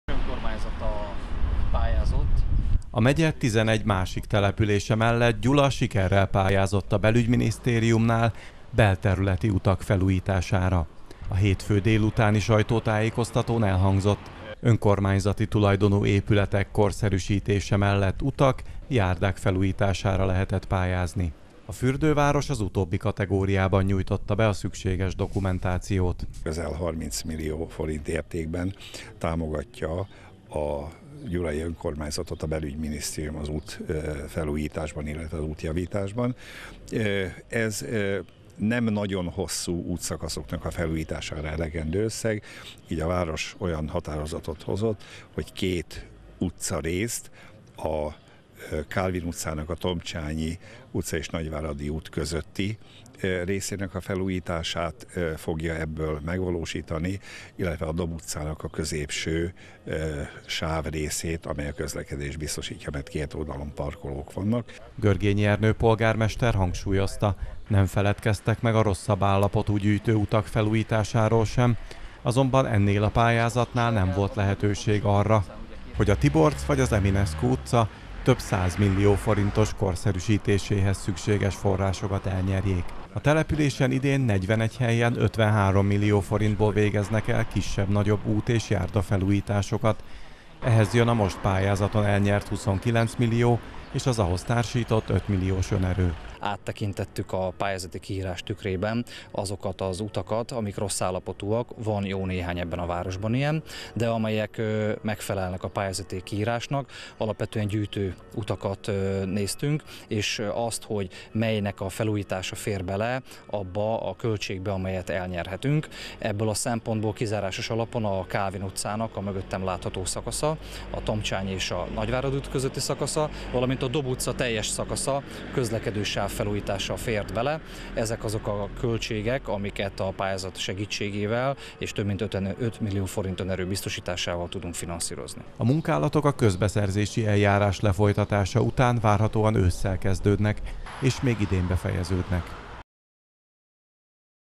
Felújítják a Kálvin és a Dob utca egy-egy szakaszát
A munkálatok ősszel kezdődnek, és még idén befejeződnek. A város összesen 29 millió forintot nyert e célra a Belügyminisztériumtól, ezt 5 milliós saját erővel tolják meg - jelentette be Kovács József országgyűlési képviselő és Görgényi Ernő polgármester.
A Kálvinon a Tomcsányi utcától a Nagyváradi útig terjedő szakasz, a Dob utcában pedig a közlekedősáv - a parkoló nem - teljes területét érintik a munkálatok.